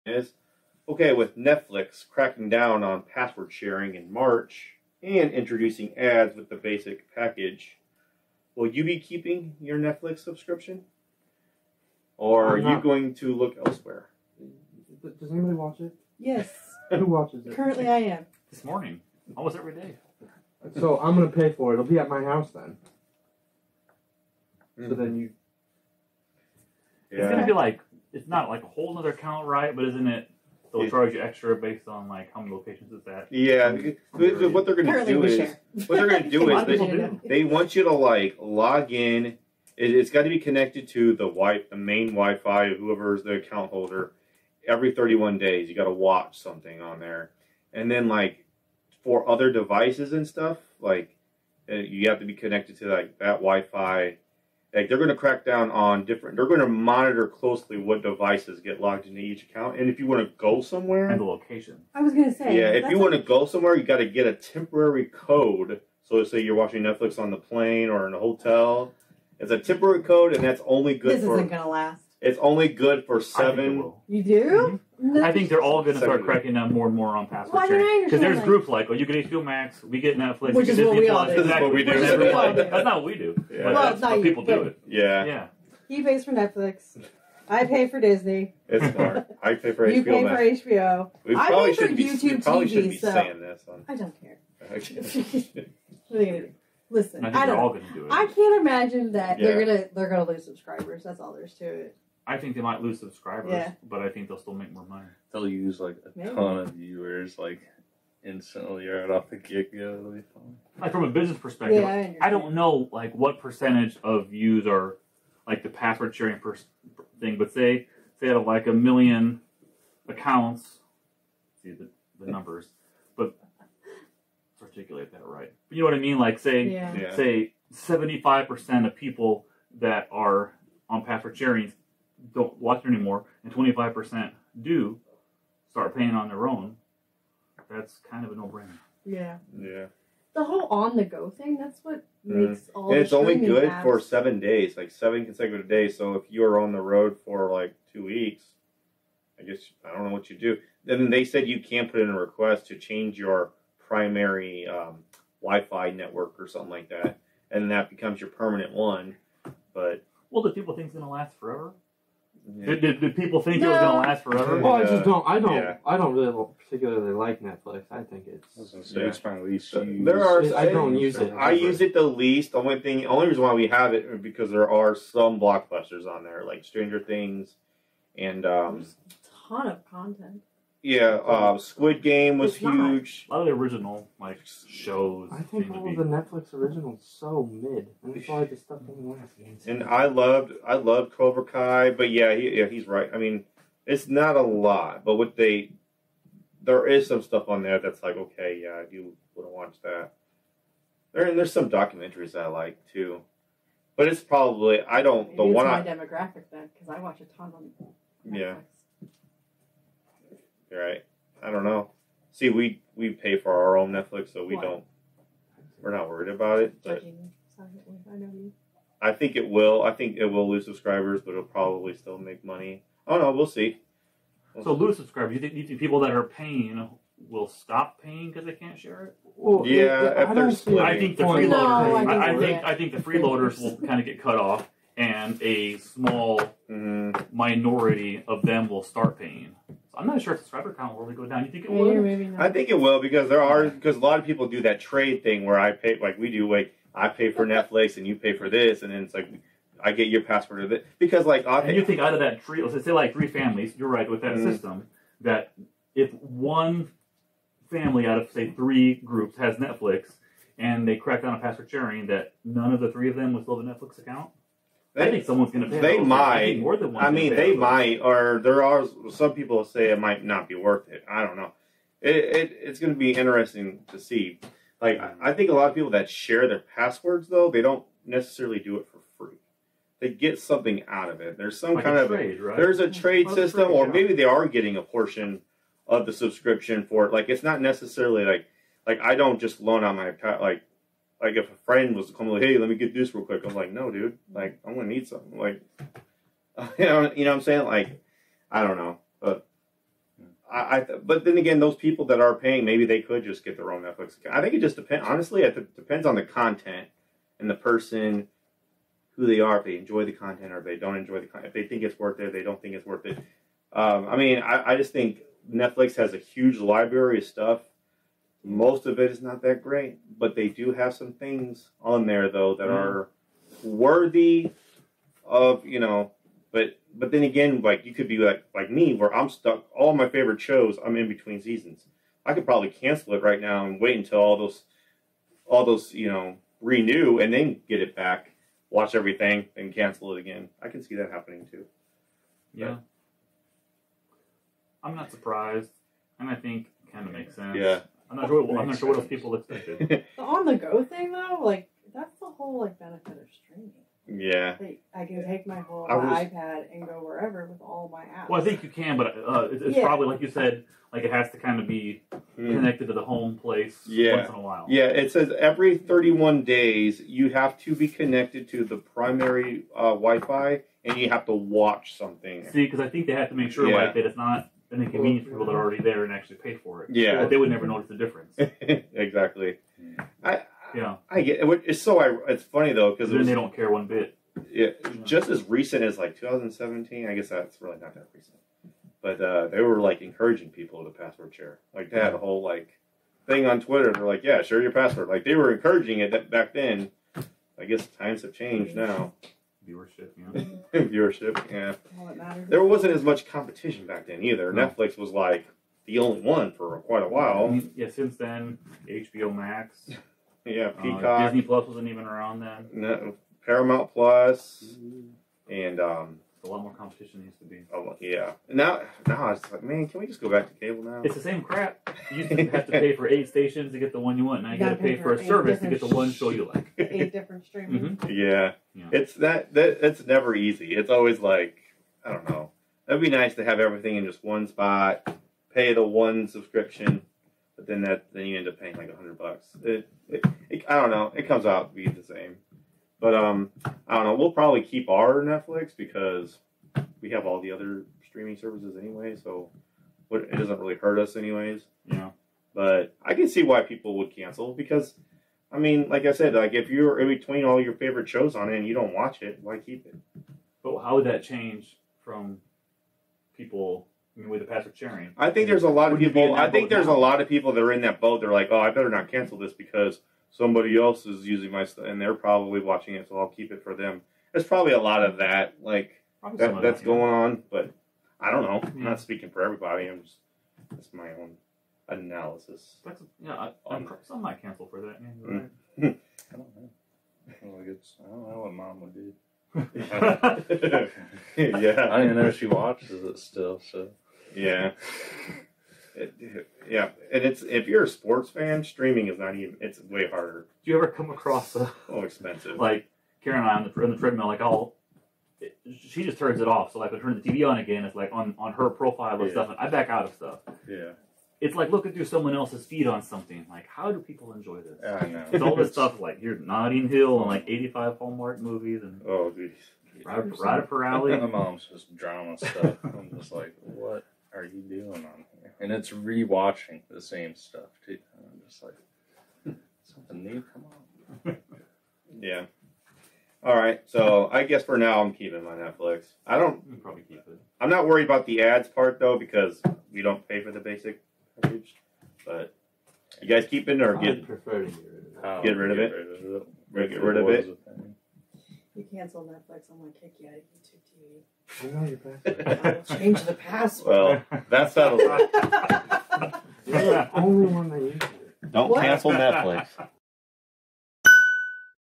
QOTD- Are You Keeping Netflix Now That They're Cracking Down On Password Sharing?
The TFD team answers Ben's Question Of The Day